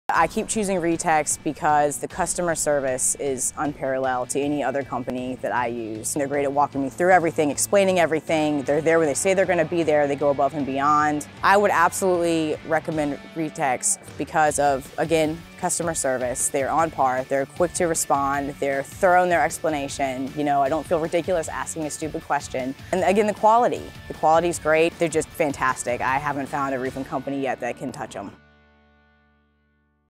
Why This Realtor Loves Working With Retex
First day of February = time to share some love for Retex, right?

Lock & Key realtor Kaylin Bradley has worked with Retex Roofing & Exteriors on multiple roofing, siding, window, and gutter projects for homeowners, buyers, sellers, and investors throughout Richmond, VA. Here's why:

"I keep using Retex because the customer service is unparalleled to any other company that I use," Kaylin says. "They're great at walking me through everything, explaining everything, they're there when they say they're going to be there, they go above and beyond.

"They're just fantastic," she says. "I haven't found a roofing company yet that can touch them."

Listen to Kaylin's experience with using Retex on multiple homes throughout Richmond while seeing how those homes' exteriors look today.

If you're a homeowner in the Greater Richmond, VA, area and you're in need of help with your roof, siding, windows, or gutters, Retex is the team to call.